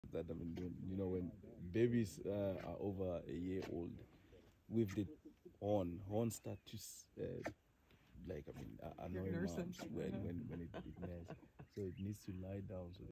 Baby Rhino nursing
Baby Rhino nursing on momma in Sabi Sands